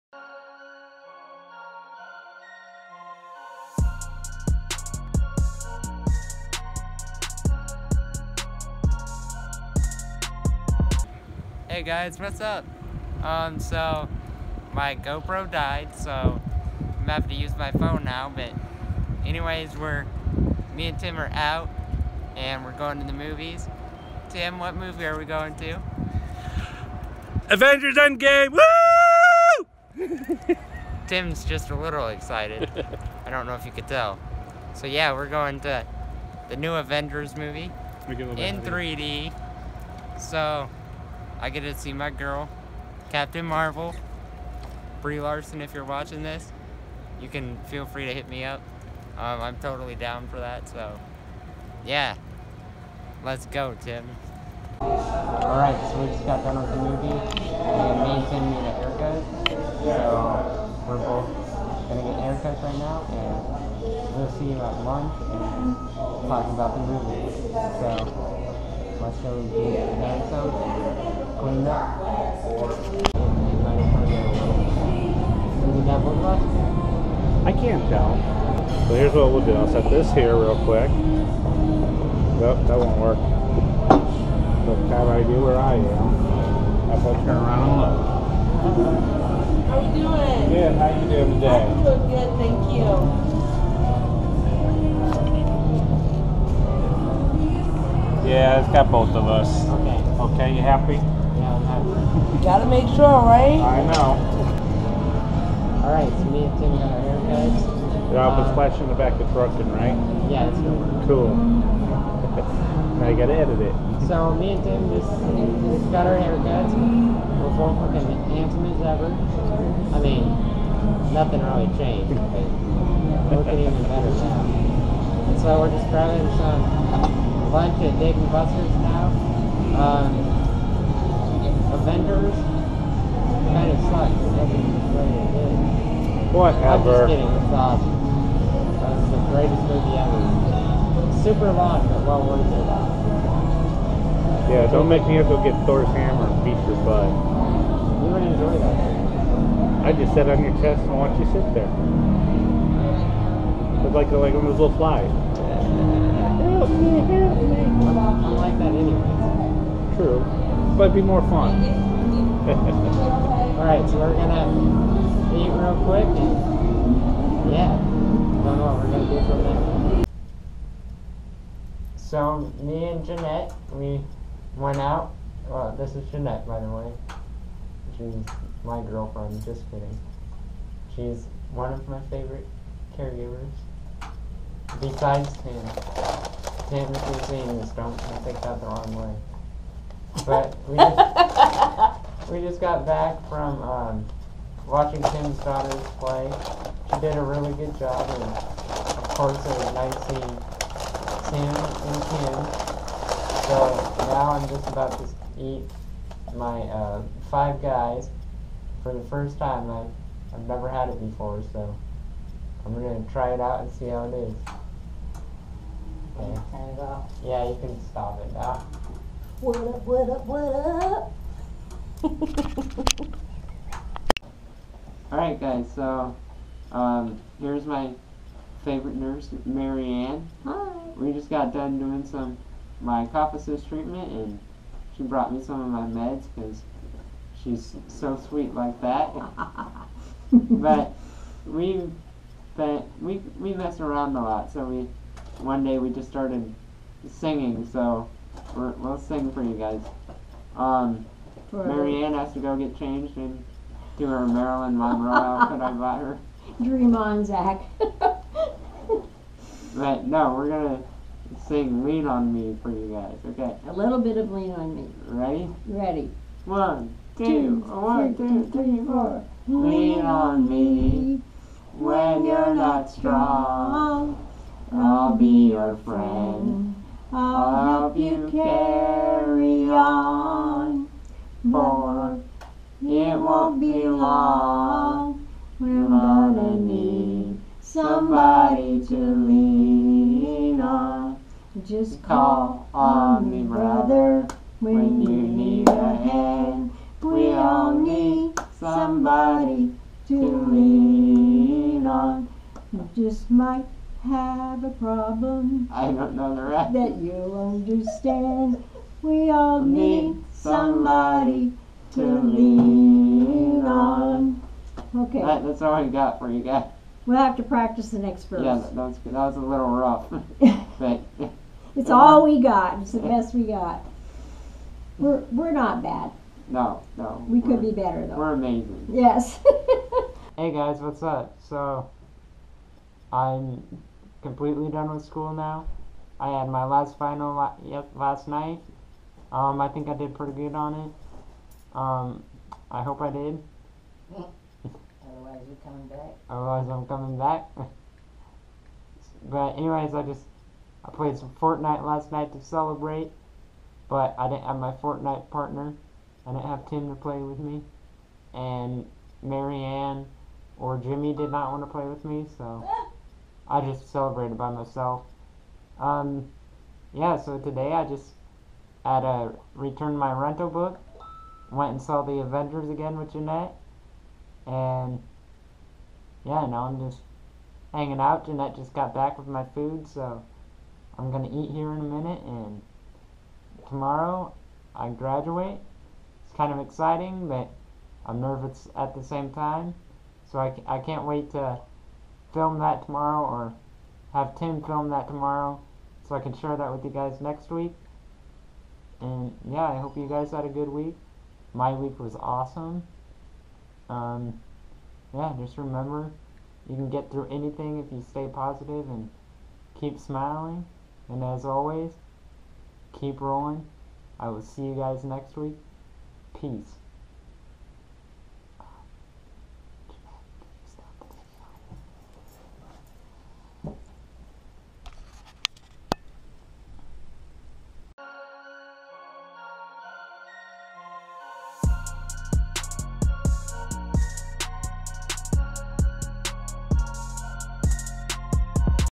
CAN’T CONTROL HIMSELF!!
Tim can’t control himself when it comes to the movies! Especially when it comes to seeing Avengers Endgame! This week was full of fun adventures and experiences, including a performance with a very special guest!

Never let anything stop you, keep that smile on your face, and keep rollin!